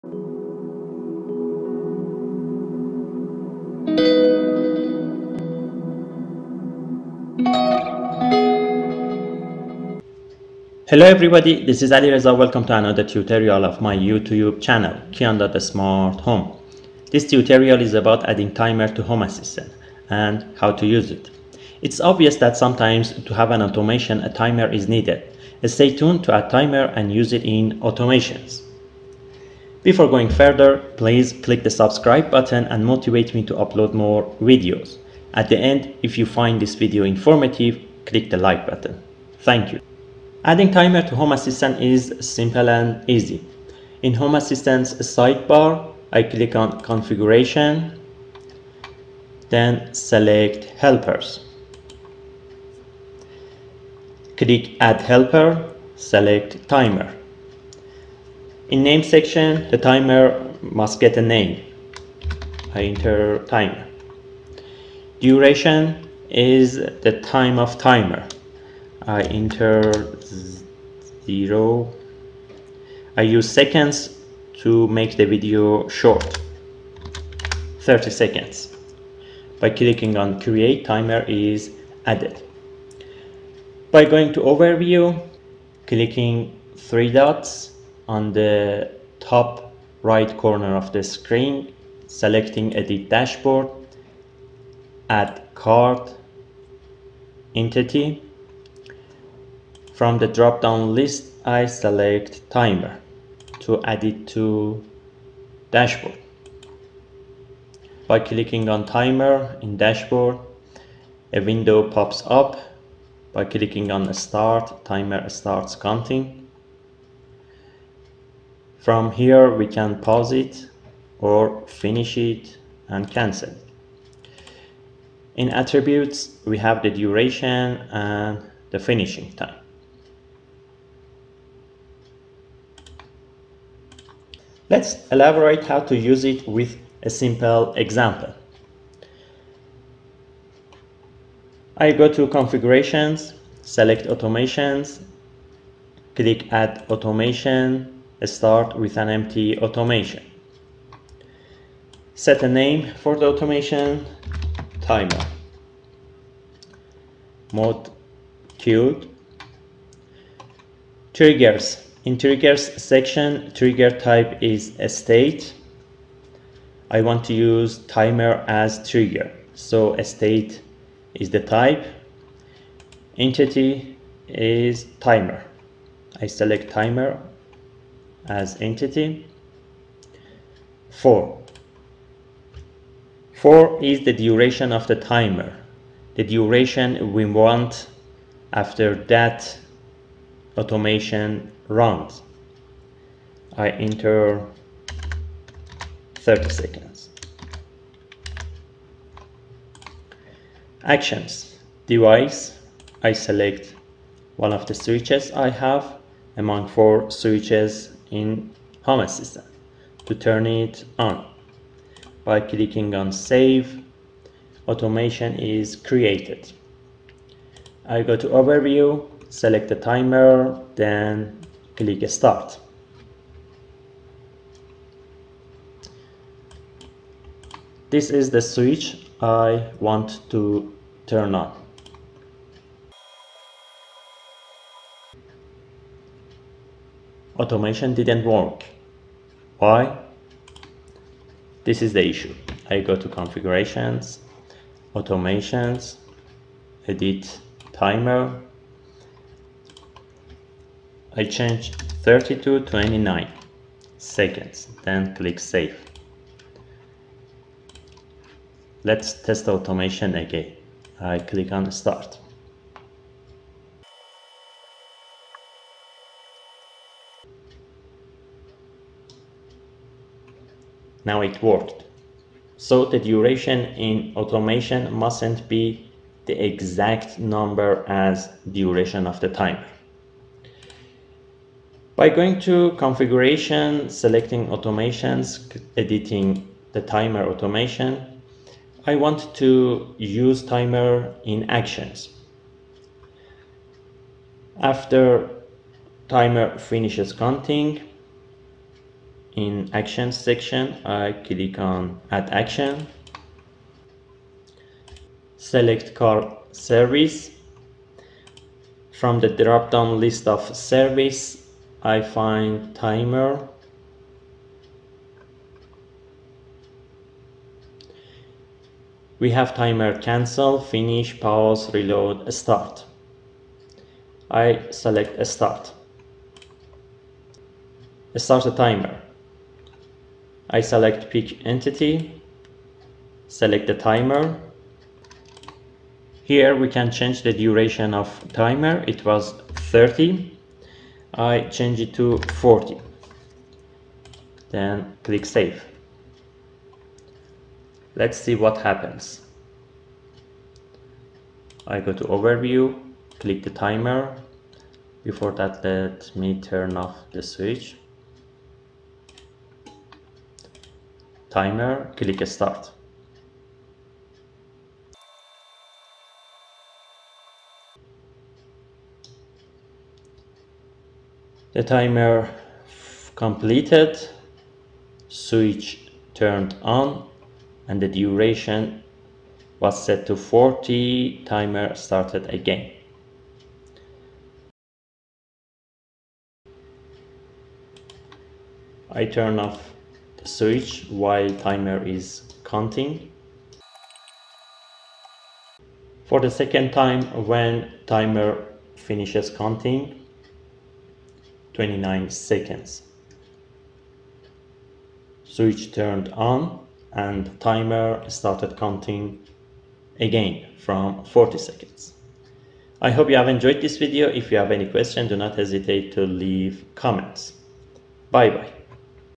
Timer in Home Assistant
Add timer to Home Assistant and use it as trigger or in actions.

00:00 beginning and introduction
00:41 Add Timer
02:13 Use as trigger
04:40 use in actions